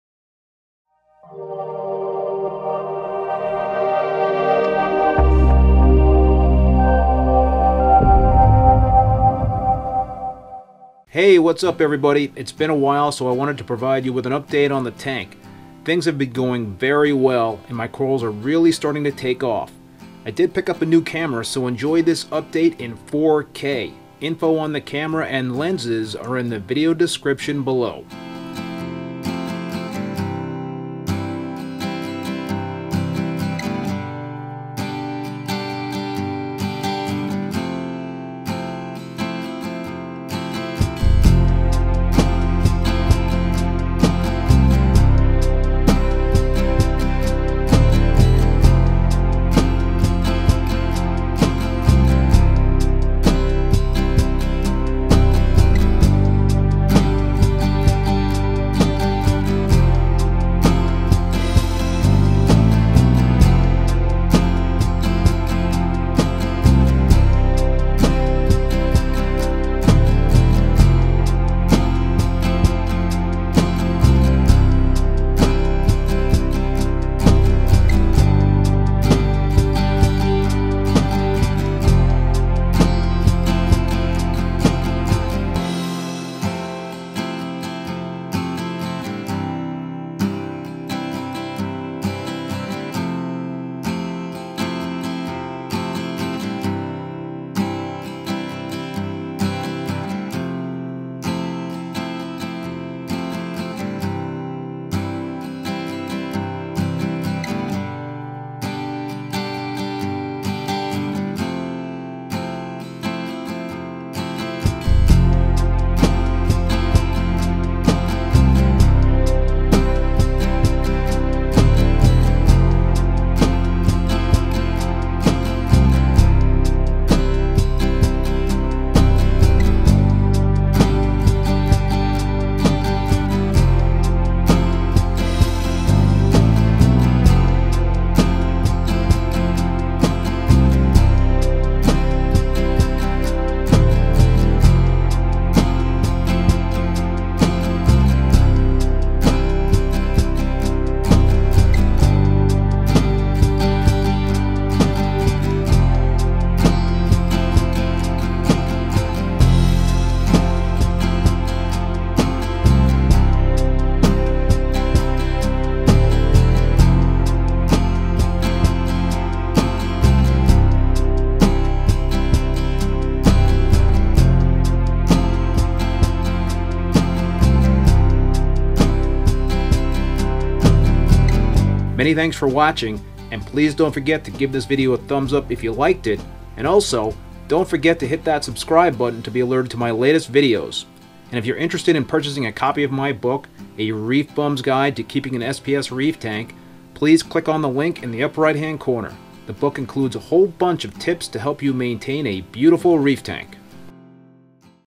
ReefBum Tank Update in 4K! - November 2018
Here is the latest on how my 187 gallon reef tank is doing. Enjoy the 4K footage!

Sony FE 90mm F2.8 Macro Lens

Sony FE 24-70mm F4

Gimbal

Zhiyun Crane